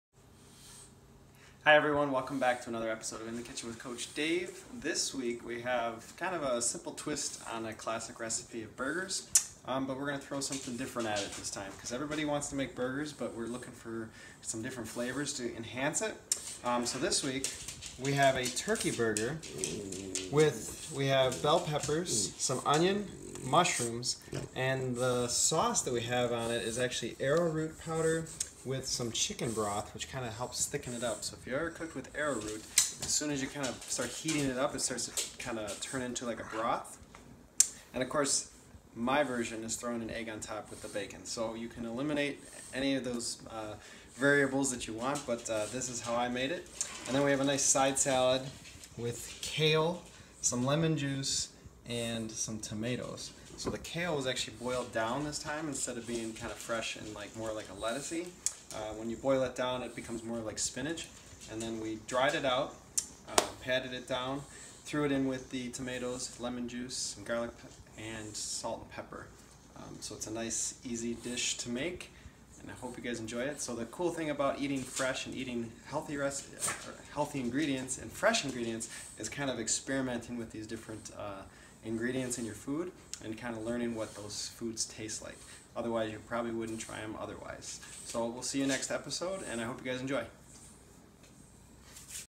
Salisbury Turkey Burgers with Chard tomato salad
Coach Dave uses arrowroot powder to make Salisbury turkey burgers and a side salad of chard and tomato’s